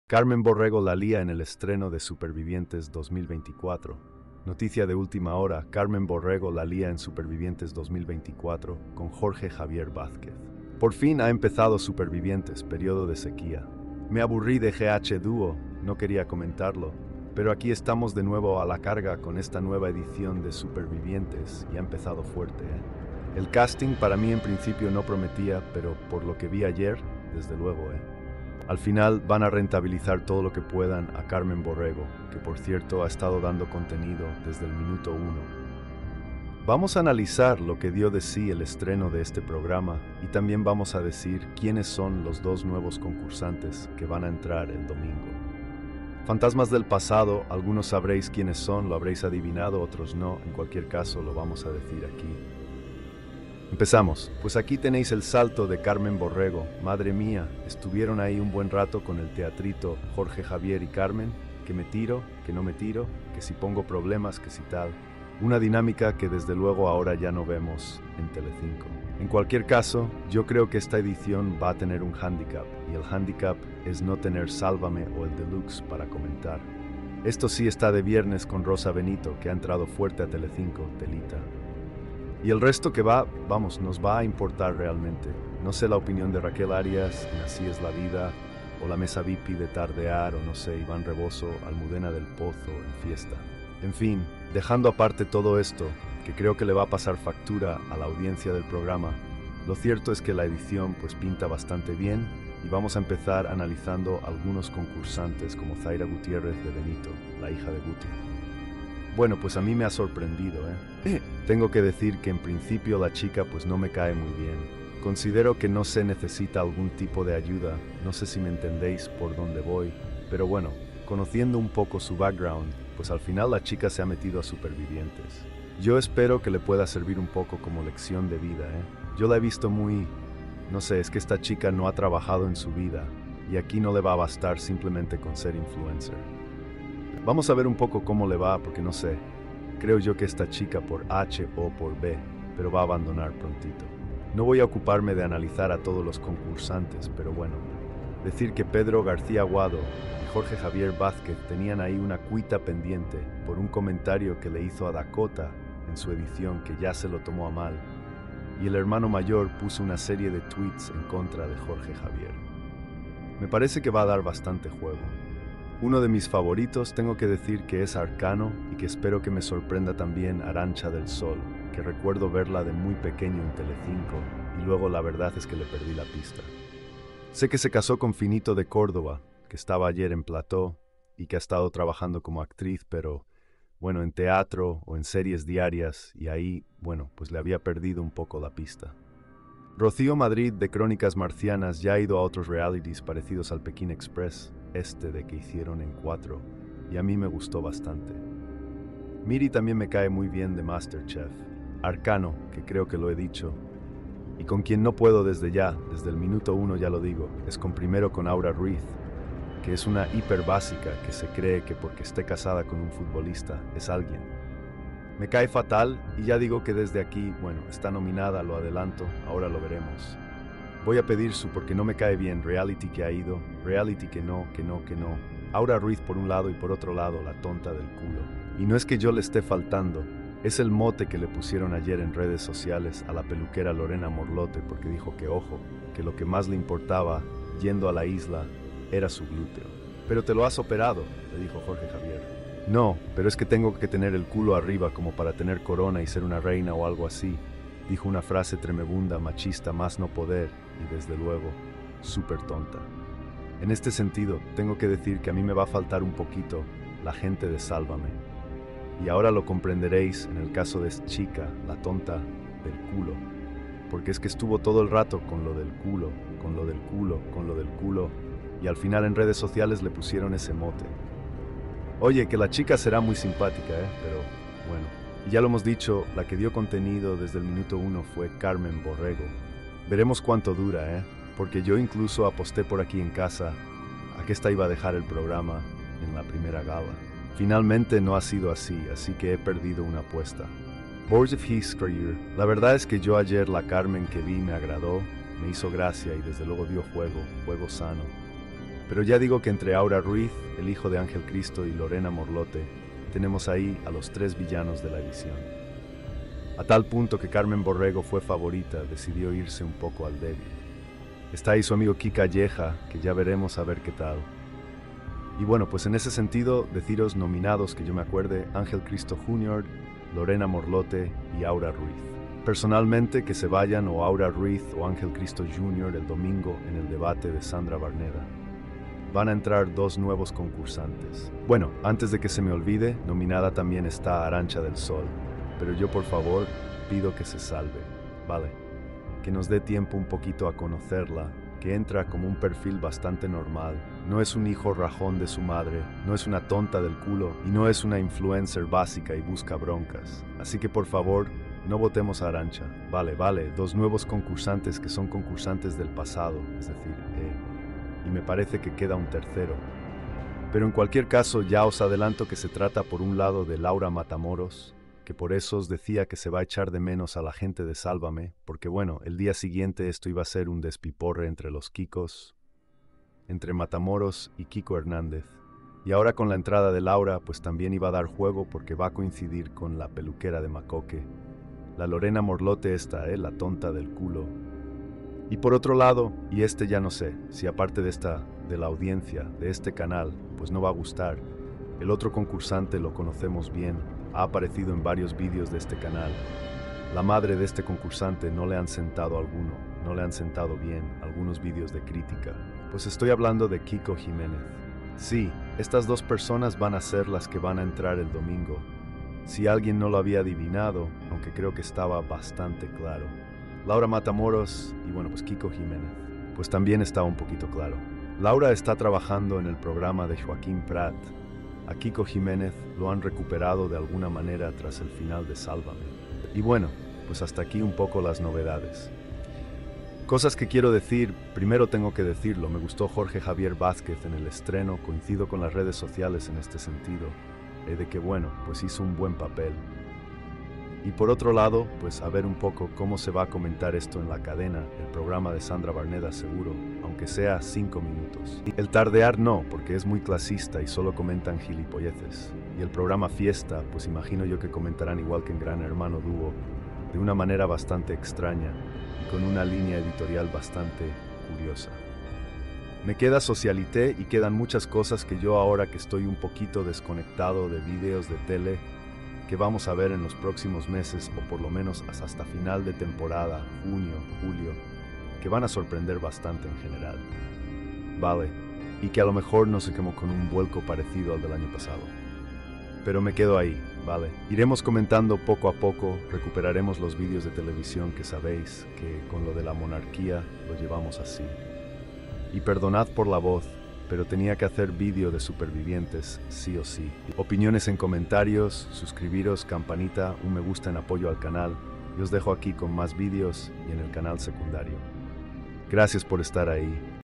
🔴CARMEN BORREGO la LÍA en el ESTRENO de supervivientes 2024
En este video impactante, analizamos cómo Carmen Borrego causa revuelo en el estreno de Supervivientes 2024. Sumérgete en este emocionante análisis de los eventos que rodean su entrada al programa y las repercusiones que esto tiene en el ambiente del reality. Acompáñanos mientras exploramos los detalles de este fascinante inicio que ha dejado a todos con la intriga y la expectativa de lo que está por venir.